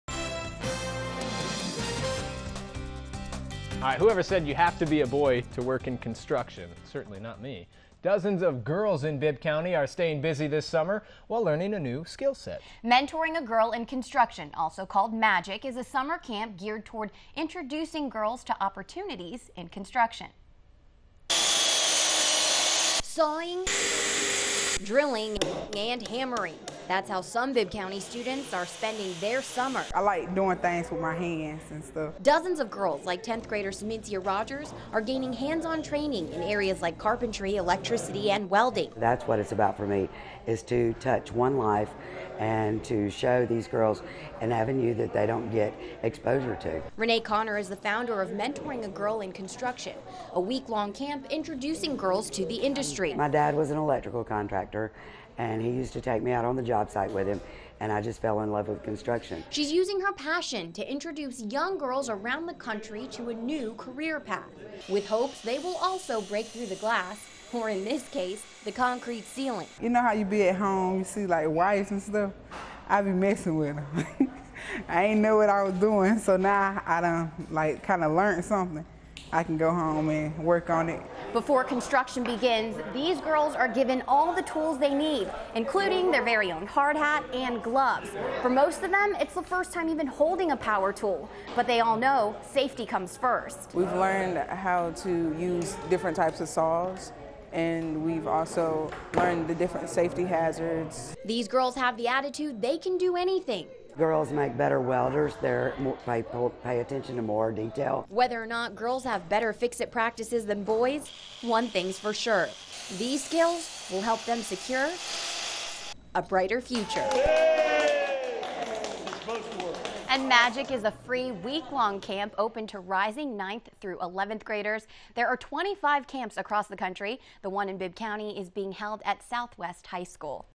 41NBC/WMGT- Girls Learn "MAGIC" in Bibb Co. Classroom - 06.06.13
Dozens of girls in Bibb County are staying busy this summer, while learning a new skill set. Mentoring A Girl In Construction, also known as Magic, Inc., is a summer camp geared toward introducing young girls to the construction industry.